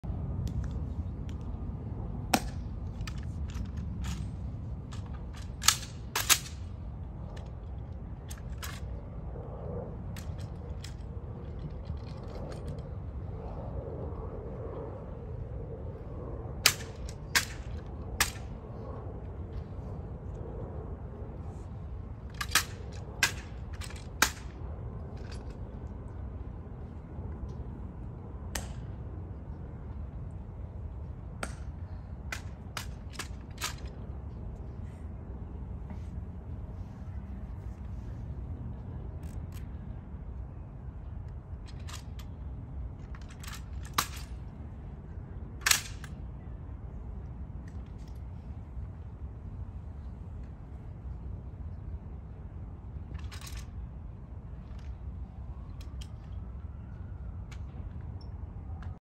Changing of the guard, tomb of the unknown soldier, Arlington cemetery unknownsoldier ￼
Changing of the guard at the Tomb of The Unknown Soldier. The tomb is guarded 365 days a year, 24 hours a day. It is a must stop on a tour of the nations capital. @LawAndCrimeNews